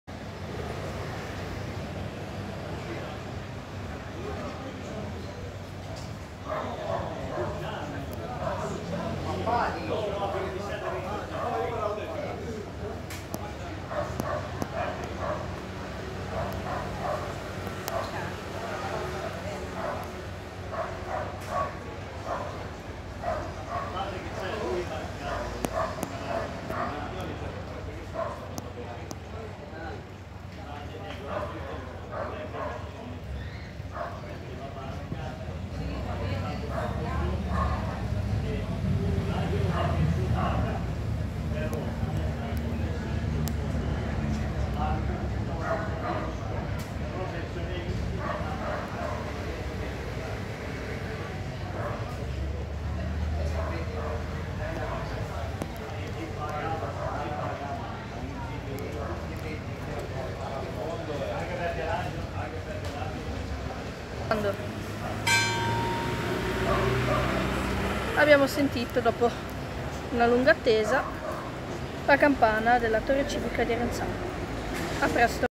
Le campane di Arenzano (GE) Palazzo di Sant' Antonio- Rintocchi orari delle 17:30
Arenzano, Palazzo Sant' Antonio
1 campana fissa in Sol4
Fusa da: ?
Rintocchi orari delle 17:30

Un altro video che volevo mettere era quello delľ ex palazzo comunale. Ha una piccola campana che sento sempre da casa mia.
A presto